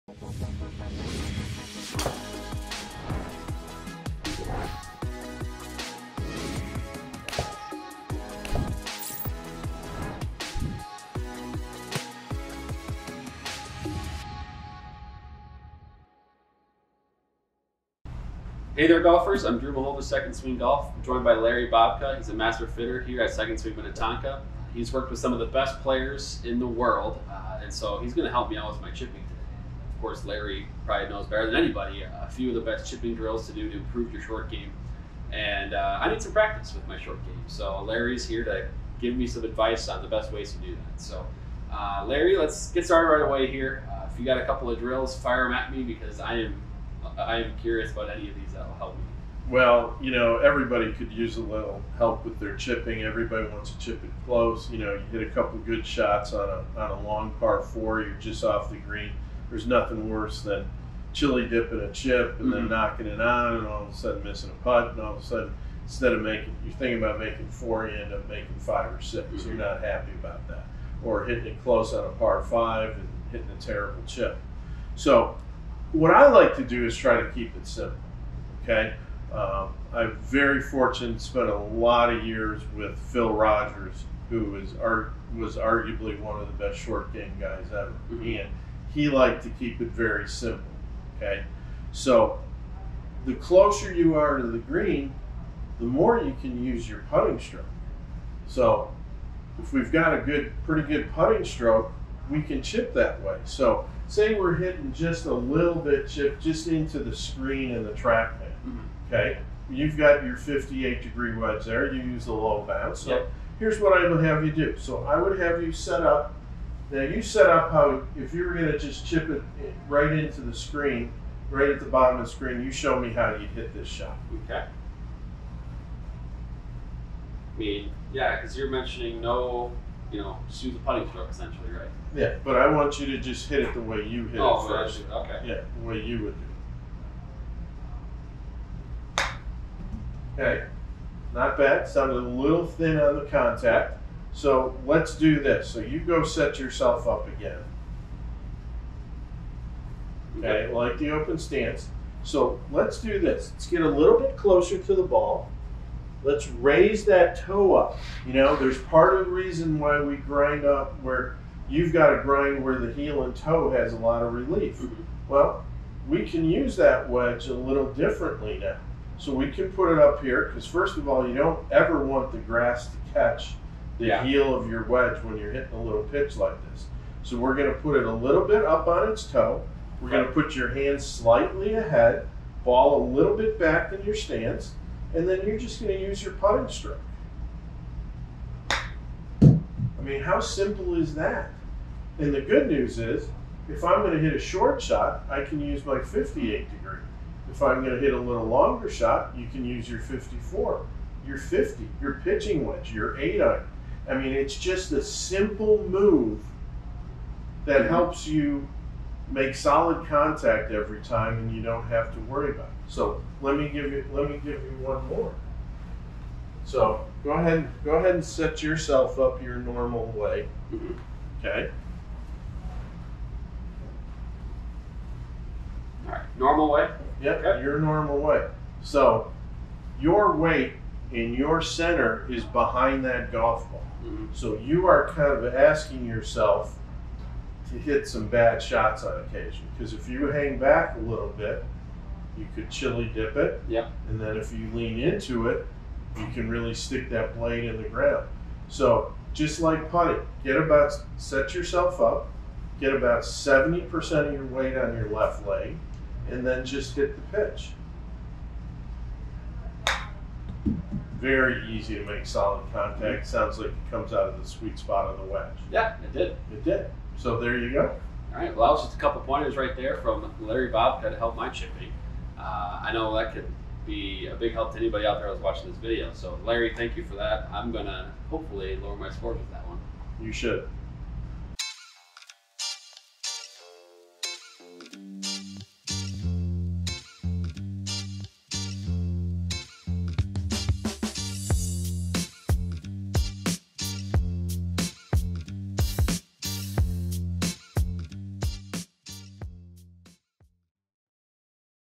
Golf Short Game Tips | Improve Your Chipping | With Larry Bobka
One of the best ways to save shots on the golf course is through your golf wedge game and your short game. Golfers face short chip shots around the green all the time, but many do not know the proper way to execute them.

In this video, 2nd Swing master fitter Larry Bobka works with 2nd Swing's Drew Mahowald to showcase a couple of pointers that will help his golf wedge game.

Schedule an award-winning 2nd Swing Tour Van fitting with Larry Bobka